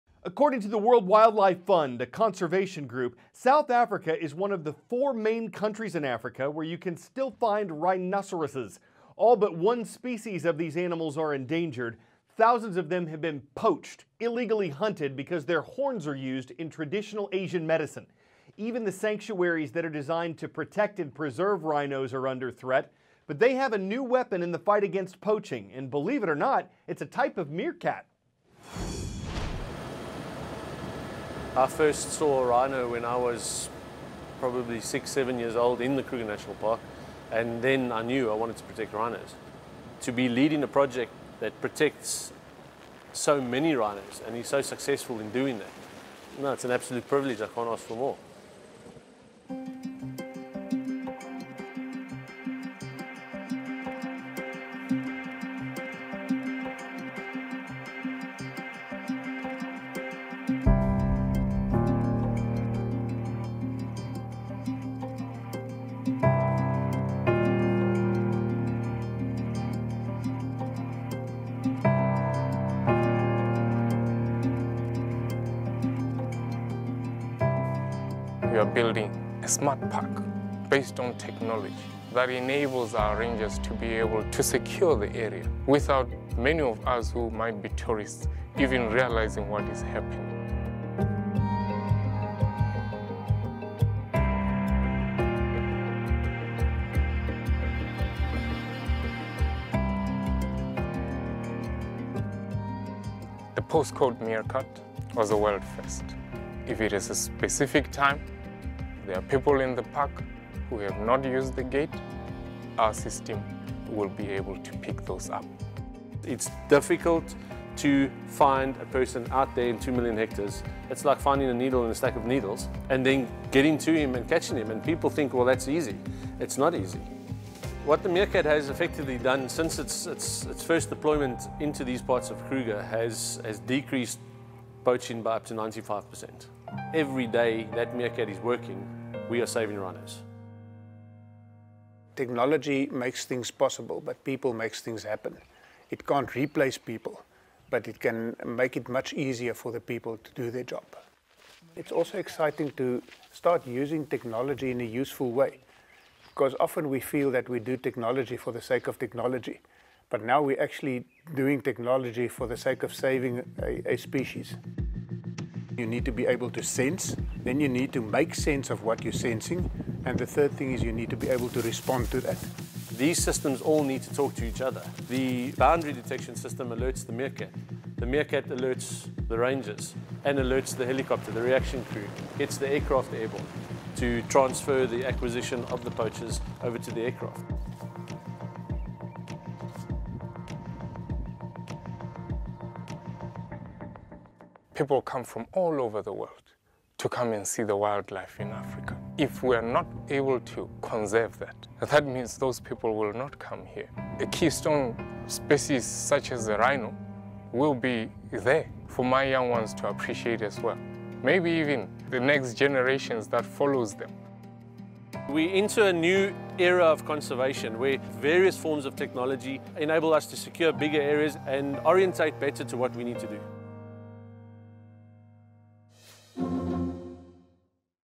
Technology To Save Rhinos
The southern white rhino has been to the brink of extinction and back, but even as it continues to be under threat from poachers, it's survival is largely thanks to the conservation work of one park.
Today, most of these rhinos can trac